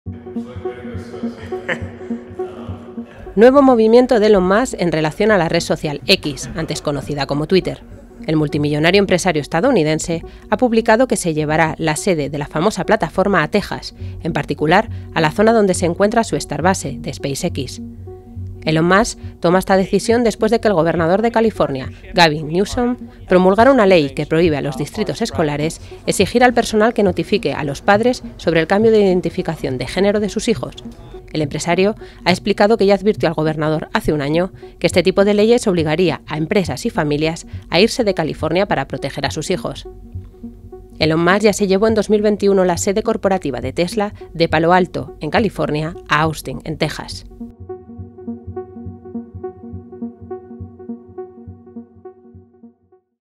Elon Musk se lleva la sede de X (Twitter) a su Starbase en Texas
Nuevo movimiento de Elon Muks en relación a la red social X, antes conocida como Twitter. El multimillonario empresario estadounidense ha publicado que se llevará la sede de la famosa plataforma a Texas, en particular a la zona donde se encuentra su Starbase de SpaceX.

Elon Musk toma esta decisión después de que el gobernador de California, Gavin Newsom, promulgará una ley que prohíbe a los distritos escolares exigir al personal que notifique a los padres sobre el cambio de identificación de género de sus hijos.

El empresario ha explicado que ya advirtió al gobernador hace un año que este tipo de leyes obligaría a empresas y familias a irse de California para proteger a sus hijos.

Elon Muks ya se llevó en 2021 la sede corporativa de Tesla de Palo Alto en California a Austin en Tejas.